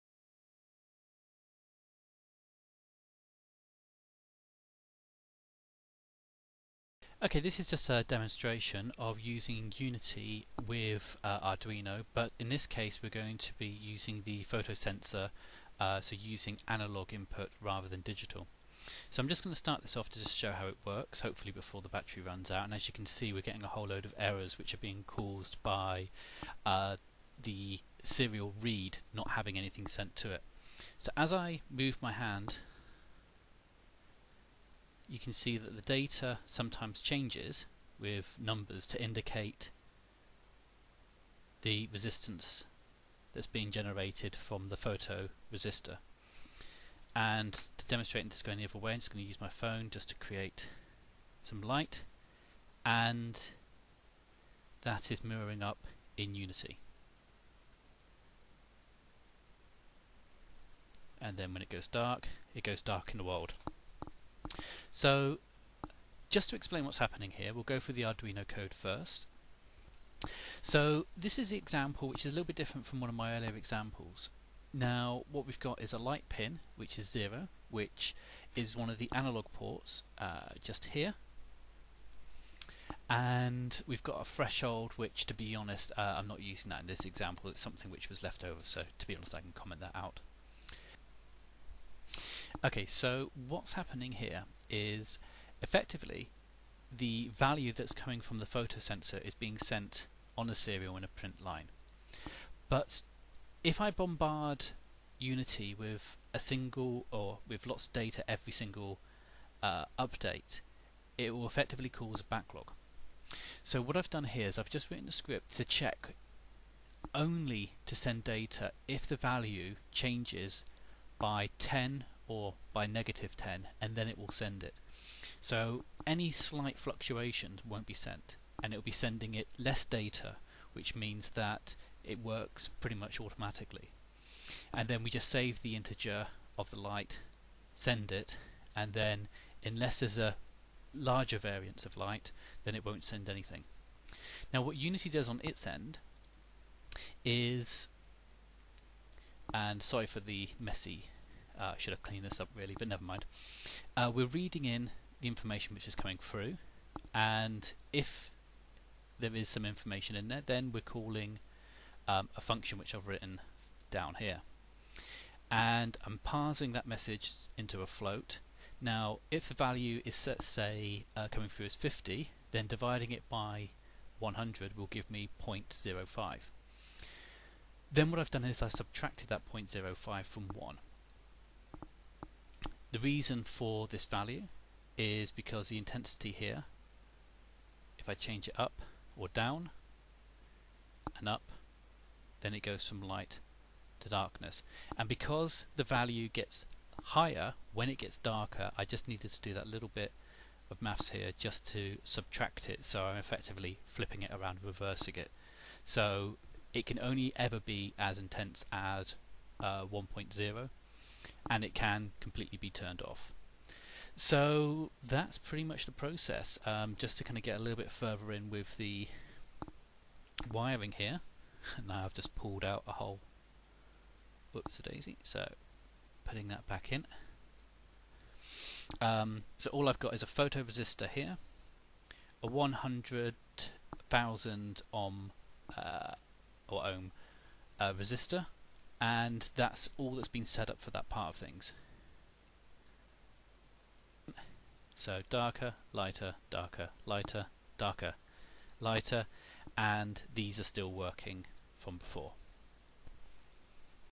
Arduino to Unity. Analogue signals from a Photoresistor.
This example is uses a simple Photoresistor to measure light and then Unity interprets the values to change the intensity on the direction light in the world.